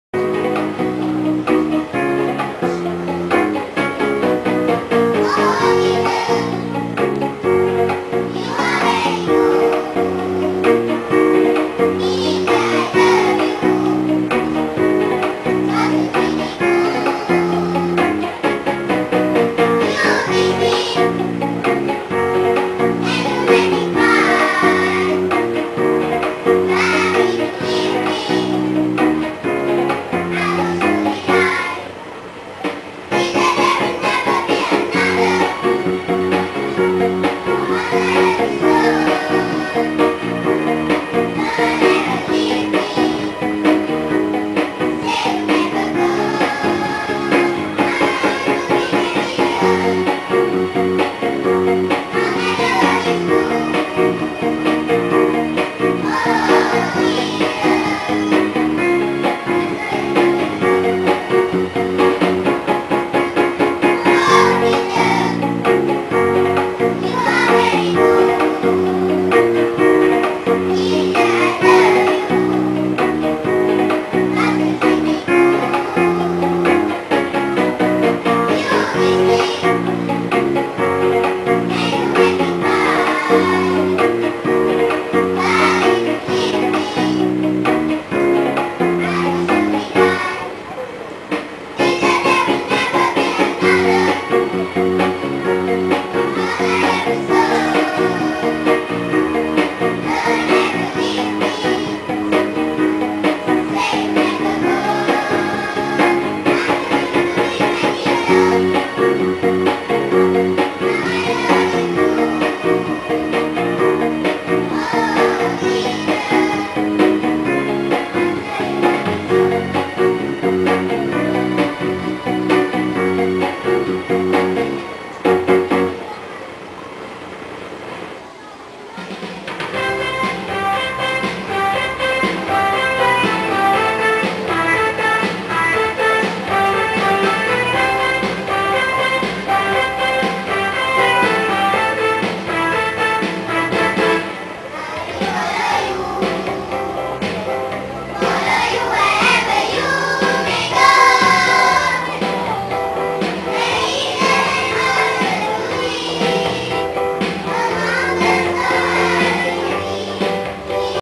Action Song Zon Kuala Pegang 2009
Peserta dari SK Syed Sheh,Kupang,Baling,Kedah dalam pertandingan action song,Karnival Bahasa Inggeris Peringkat Zon Kuala Pegang 2009. Majlis berlangsung di SK Tawar pada 8 April 2009 bermula jam 2.30 petang dan berakhir jam 5.30 petang.